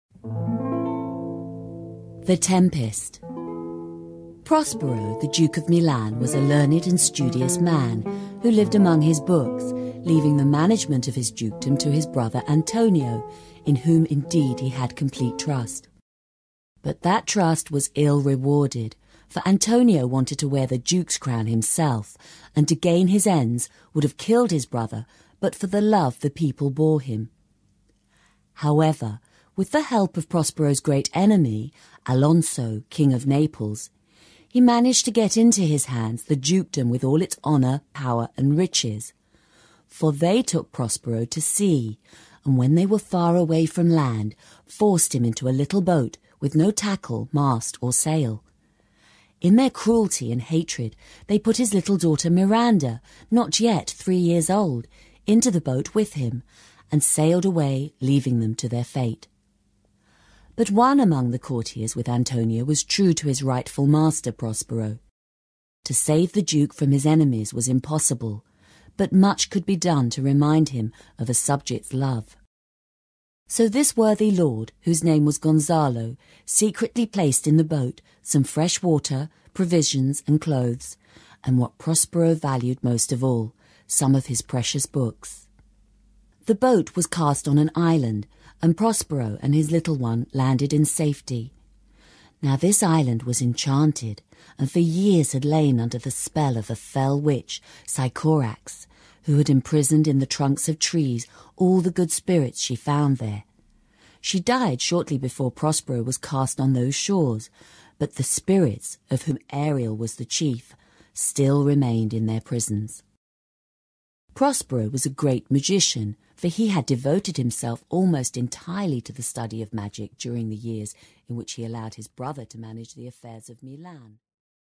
The Story of Shakespeare's The Tempest (Sample)
Read by Ghizela Rowe.

THE TEMPEST has the ingredients of a cracking good story – magic, power and love, that kick up a storm.  The magician Prospero and his daughter Miranda are banished by his jealous brother Antonio with the help of Alonso, the king of Naples.  Prospero conjures up a tempest than ensures justice is done.